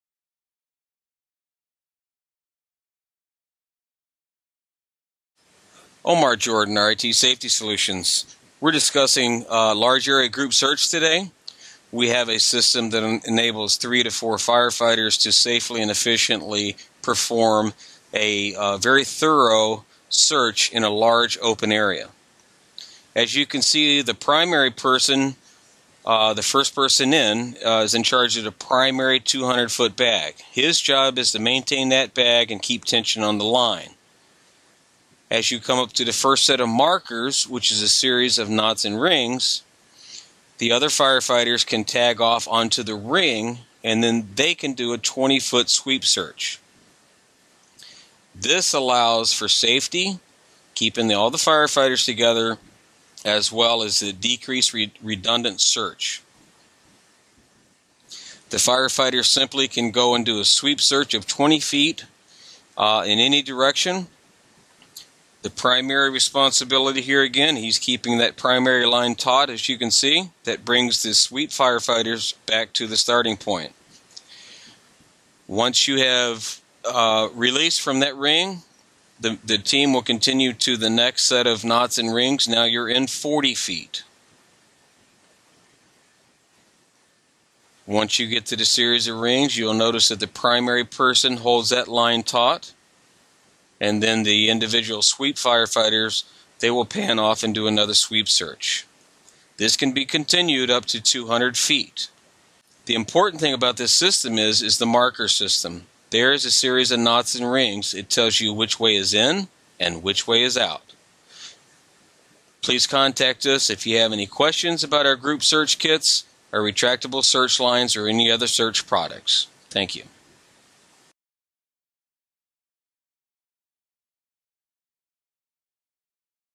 Large Area Group Search
RIT's Group Search Kit makes large area search and RIT team operations a safer task. The standard kit is set up for a four person team and provides a lifeline to the outside for each member. For searching a large area the optional marker system (knots and rings) assist in performing a systematic search and prevents duplication of search areas. The knot and ring system assures team members of their location and exit direction. RIT operations are also supported with this system. While searching for one of our own we have a tendency to search harder and longer than normal. Each RIT team member is not only connected to the outside, but the system also leads secondary RIT teams to their location for assistance and/or supplies.